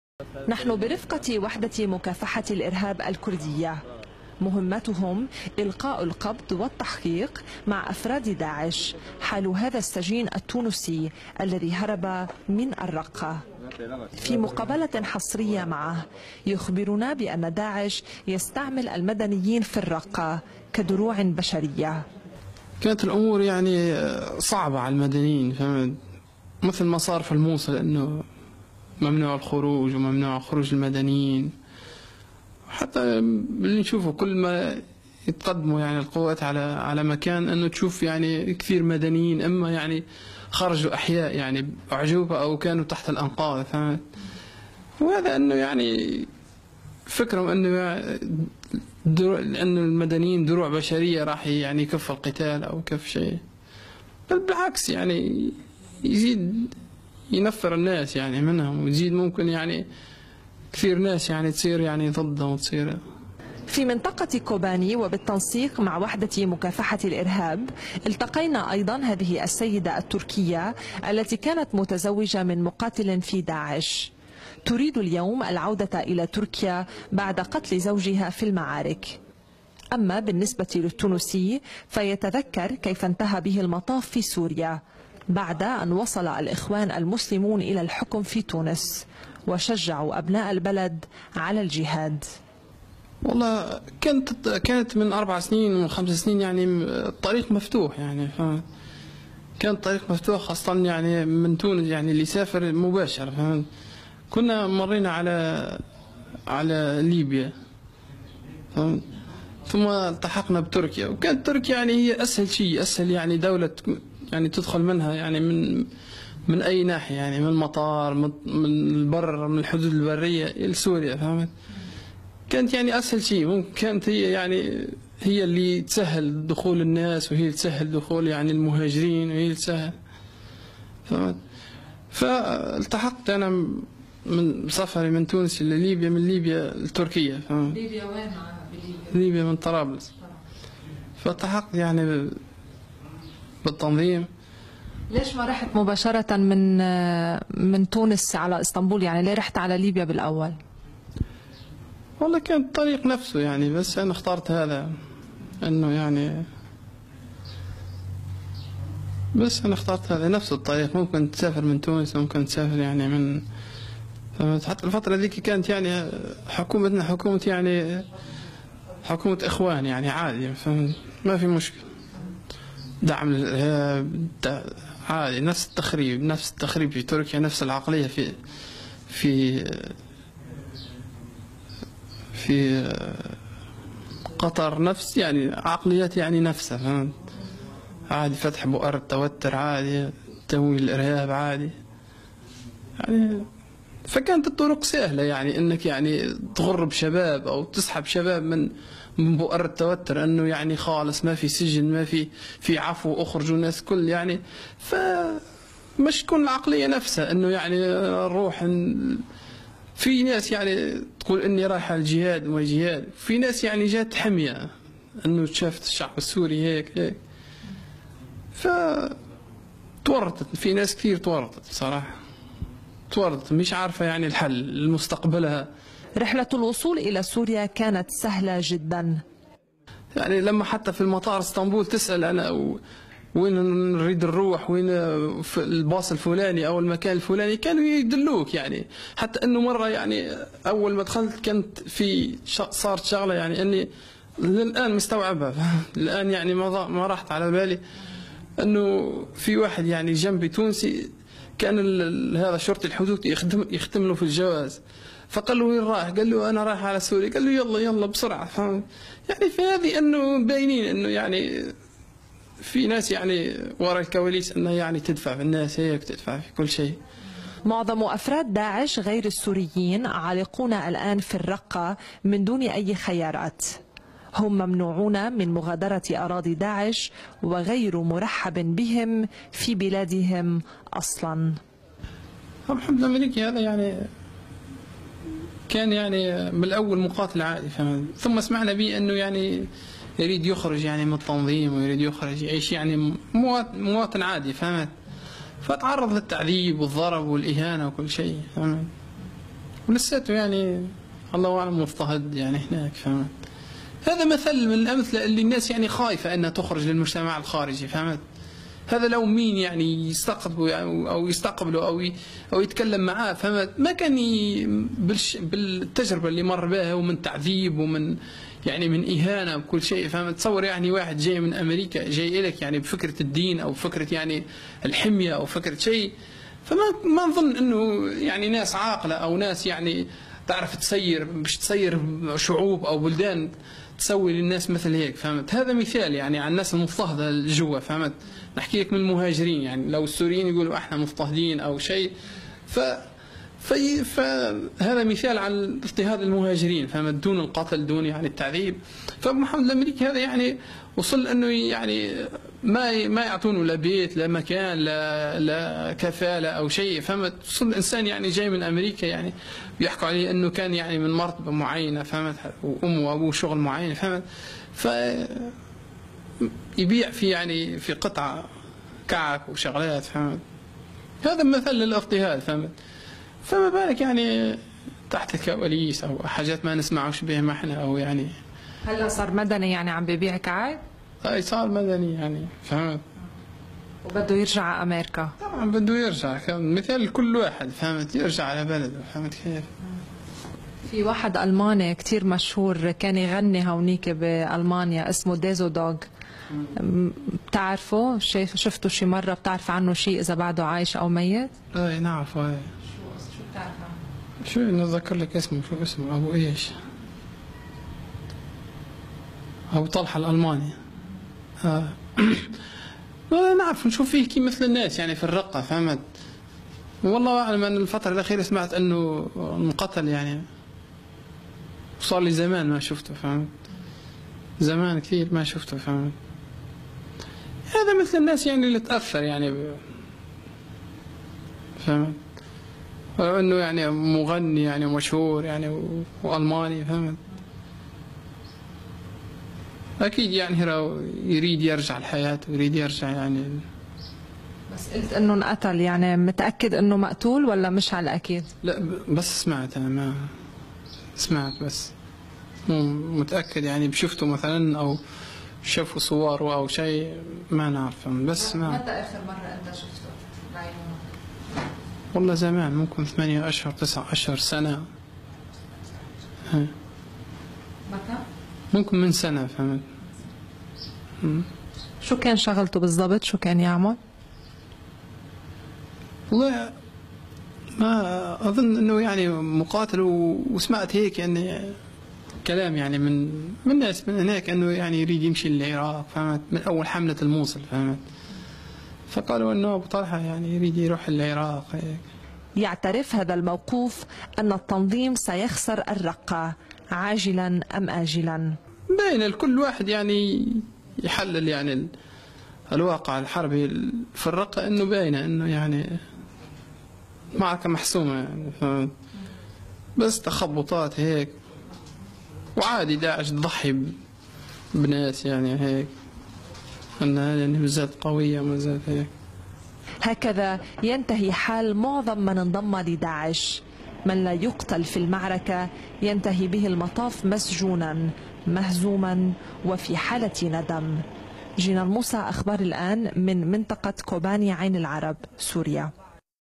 إرهابي تونسي هارب من الرقة
الإخوان في تونس شجّعونا على الجهاد والطريق إليه كان مفتوحا